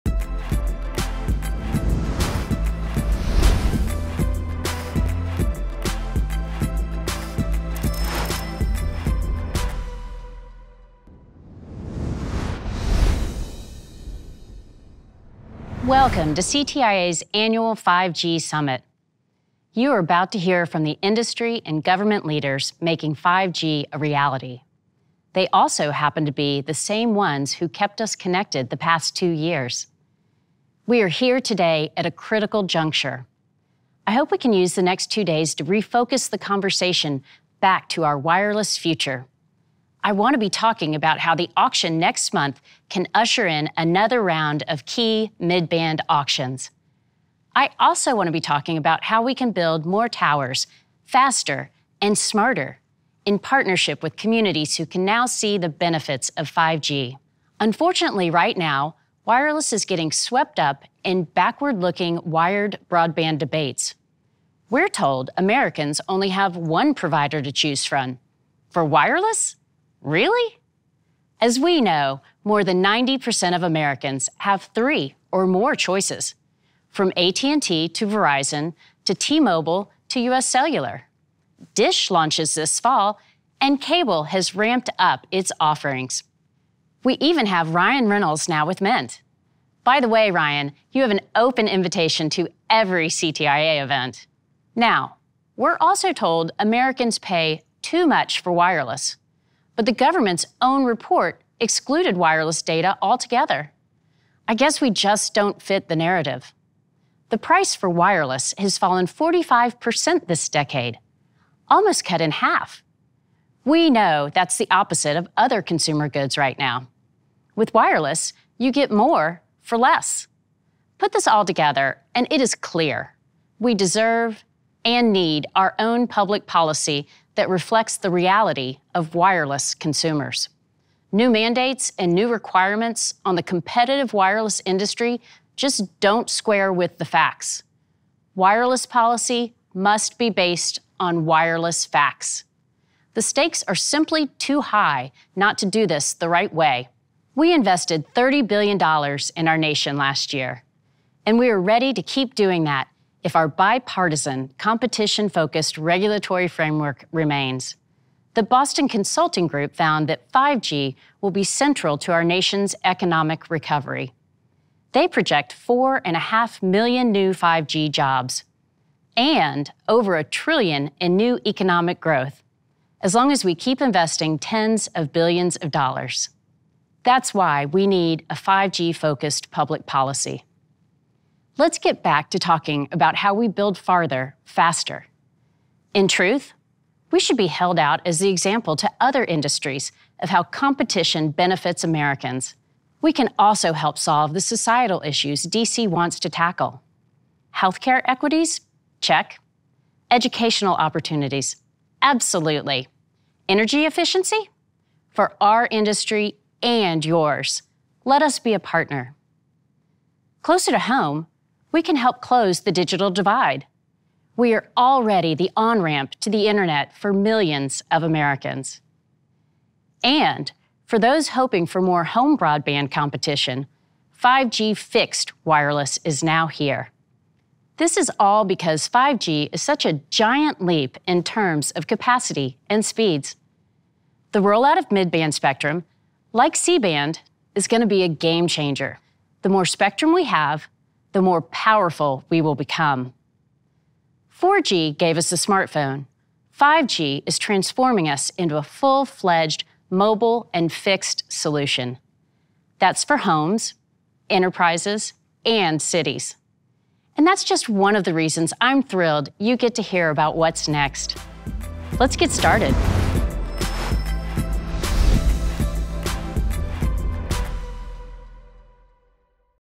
2021 5G Summit Keynote | CTIA President & CEO Meredith Attwell Baker
CTIA President & CEO Meredith Attwell Baker delivers her opening remarks and kicks off the 5G Summit on September 29, 2021.